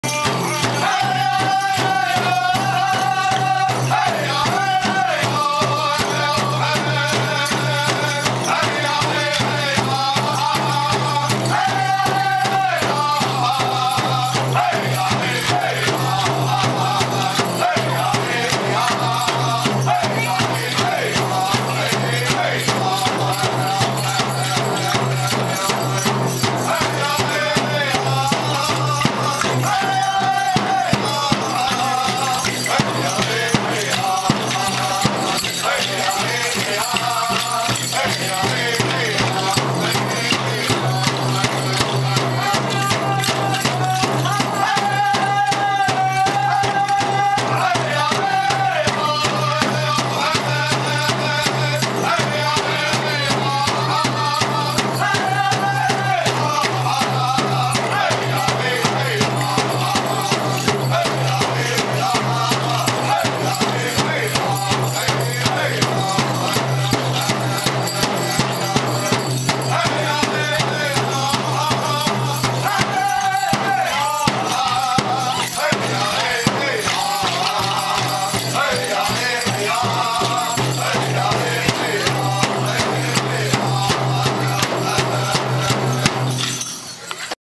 Charlotte Museum Of History Native American Dancing 11/13/10
A sweet little girl in yellow dancing alongside full-grown men :) she was having a great time. This was filmed at the Charlotte Museum of History.